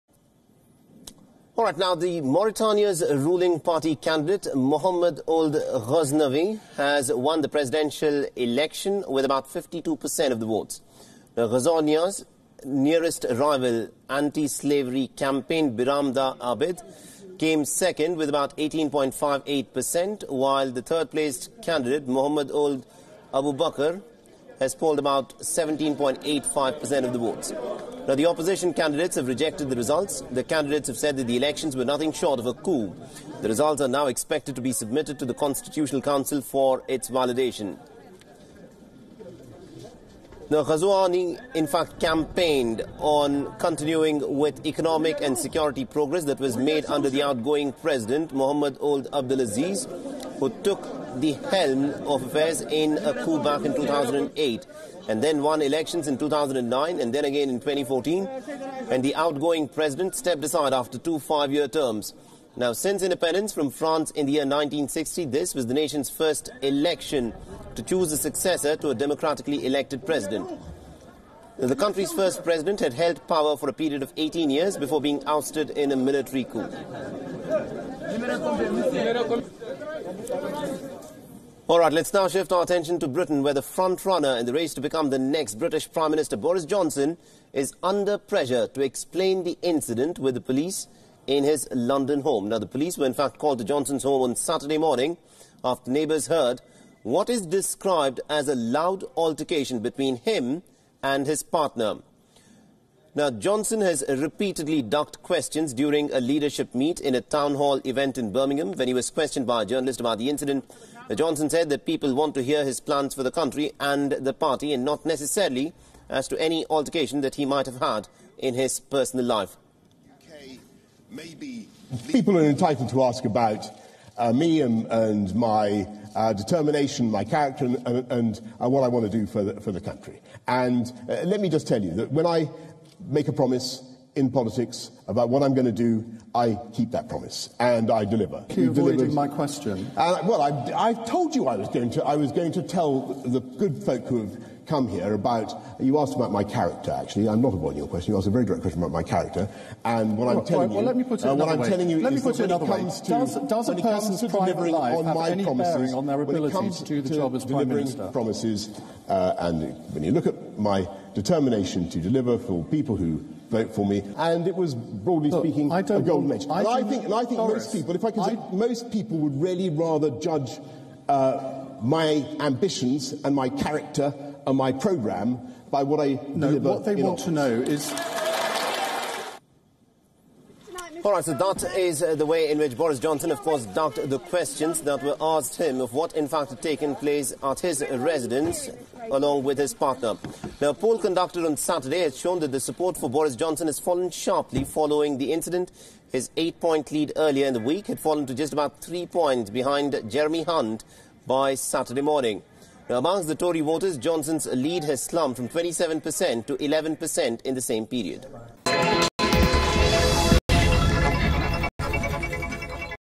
Mauritania's Ghazouani declared presidential vote winner
Mohamed Ould Ghazouani has won Mauritania's presidential election with an absolute majority.